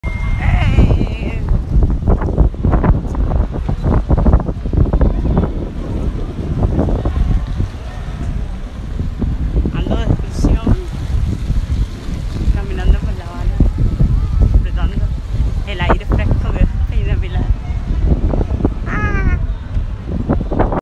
Walkthrough of my Havana trip
If you're interested in learning more about my recent trip to Havana, be sure to watch this short walkthrough video! In this video, I'll show you around the Old Havana district and discuss some of my favorite places to visit.

If you're curious about my impressions of Havana, this is the video for you! In this video, I'll give you a brief overview of my trip and some of my thoughts on the city. I hope you enjoy watching it as much as I enjoyed filming it!